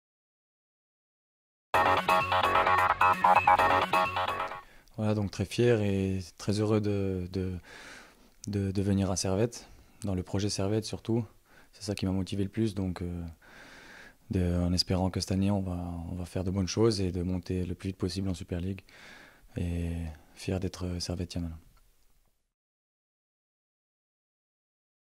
Bienvenue Sébastien Wüthrich
Sebastien Wüthrich rejoint le Servette FC !